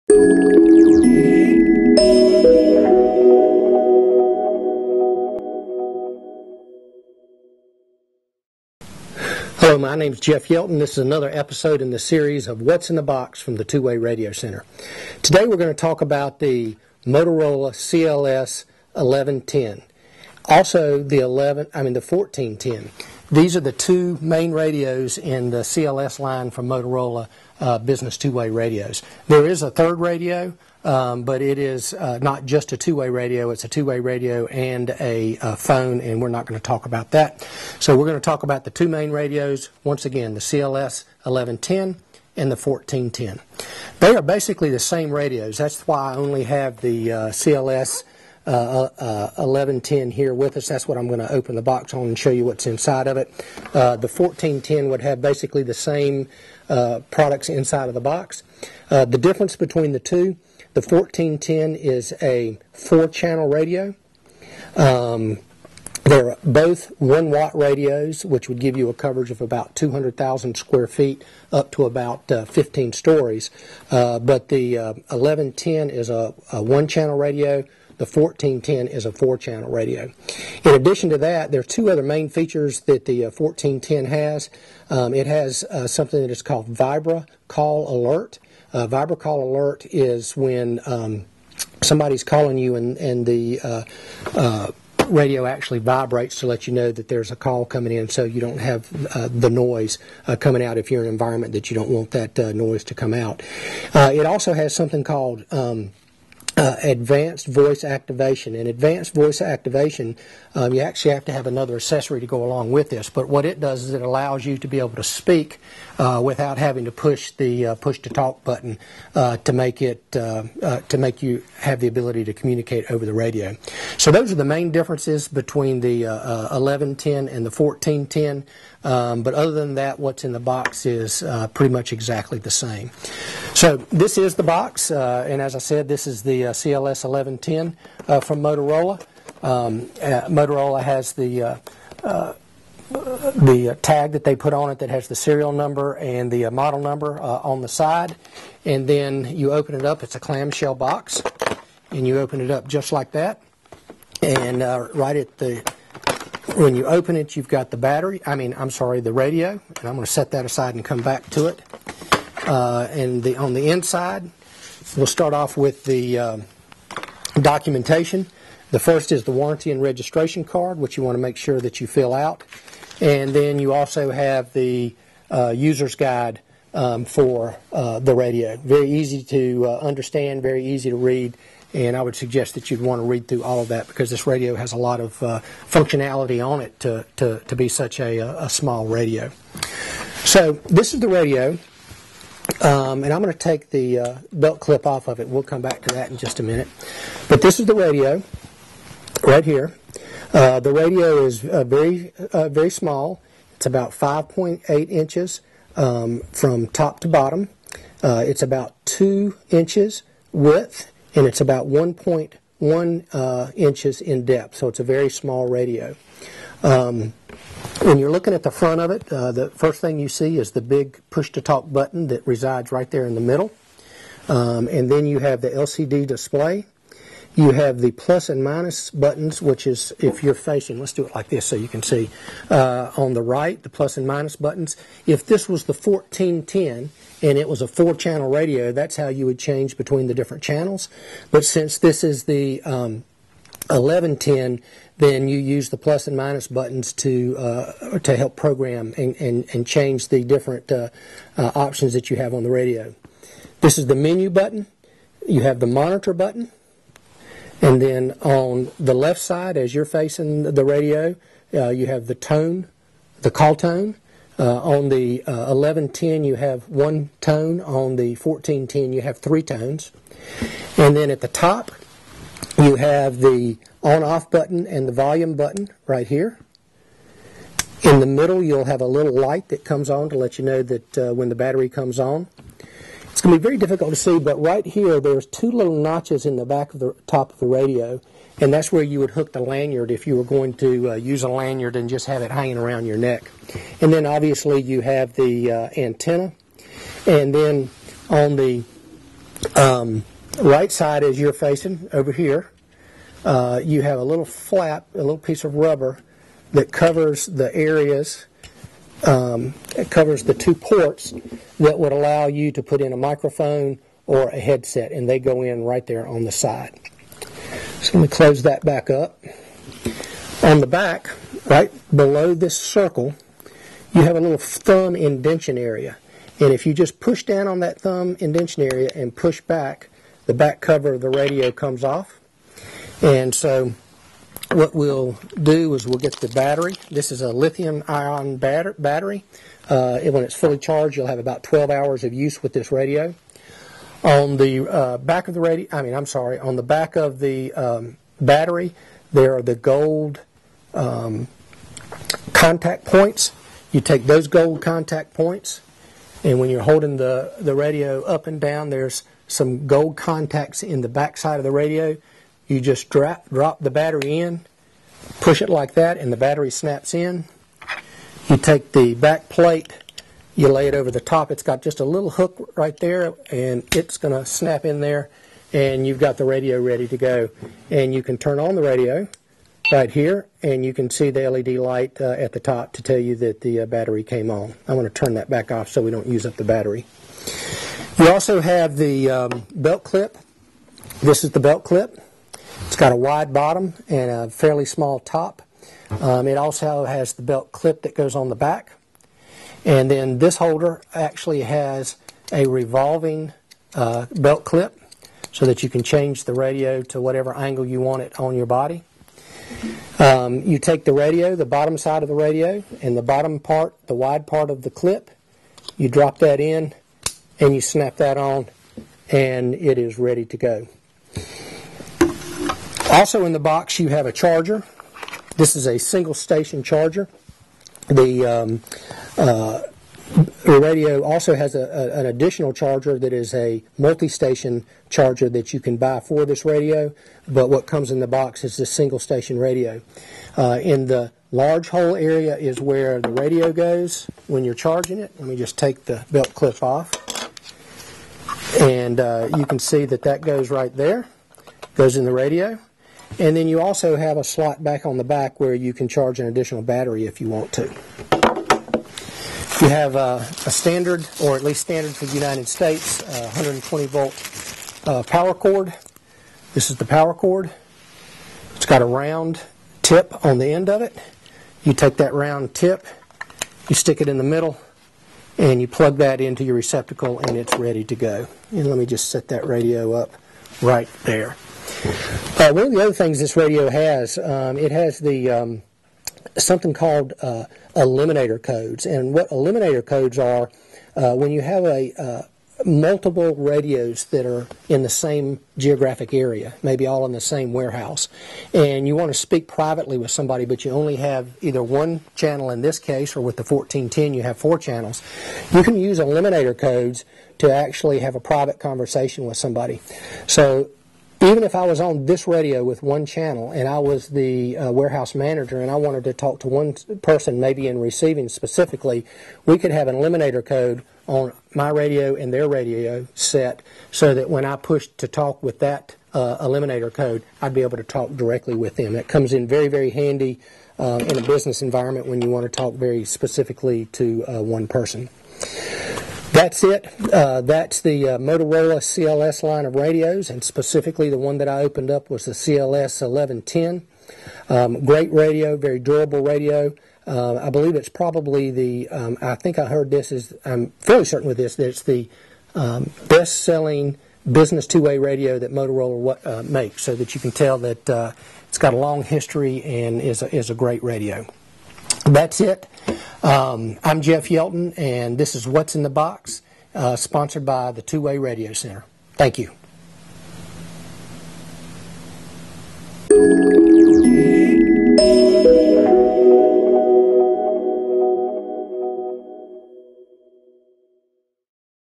Motorola CLS 1110 What's in the box
What's in the box when you buy a Motorola CLS 1110 or 1410 two way business
radio. This video shows you everything that comes in the box of the Motorola CLS1110 and CLS1410 radio.